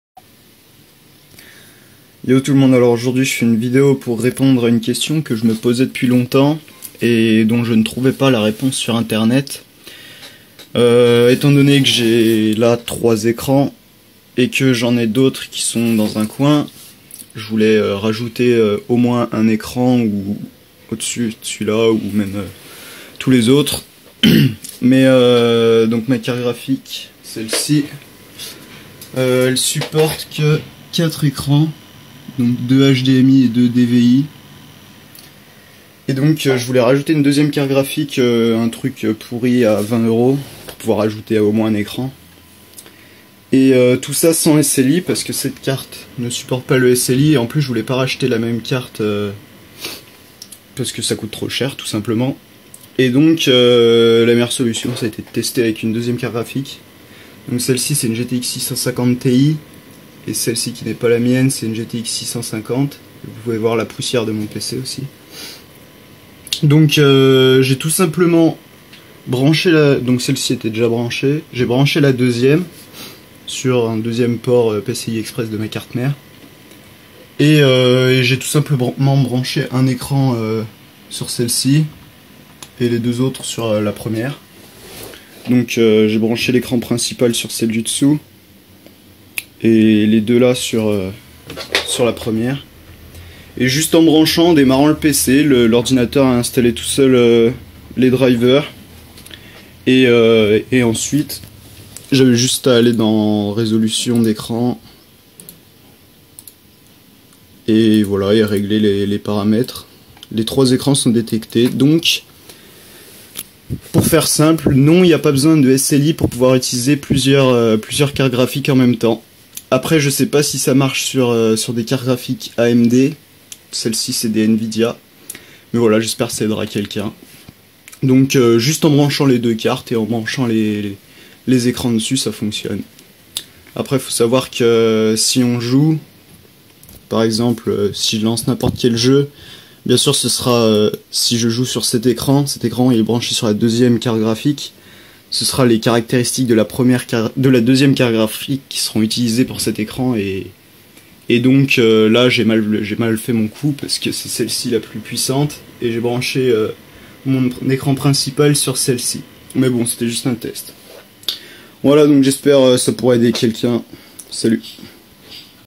2 carte graphiques sans SLI ? oui possible.. facile
Juste une petite vidéo a la va vite pour montrer qu'il est possible de mettre de carte graphique nvidia sans sli pour augmenter le nombre d'écran